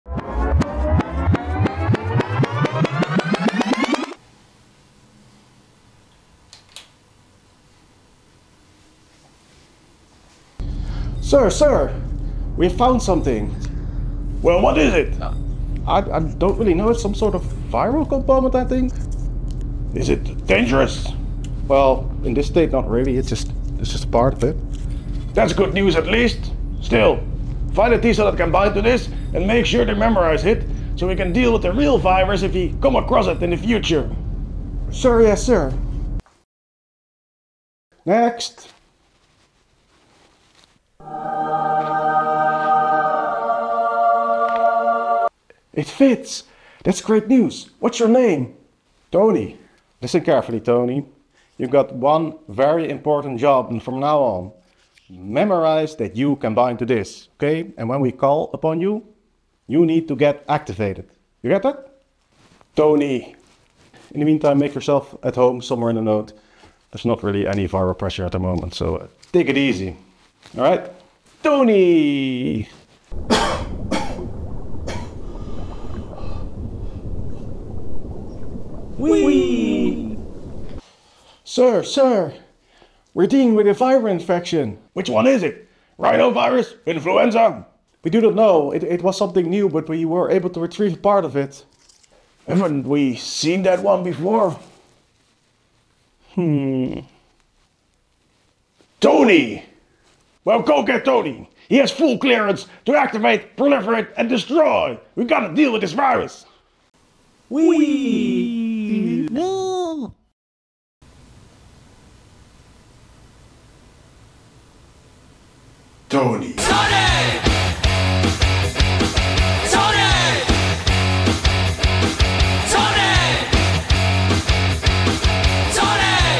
With Vaccines - Why you should vaccinate
Help your cells, help others, get your vaccinations.
Check out what happens WITHOUT vaccines in the full video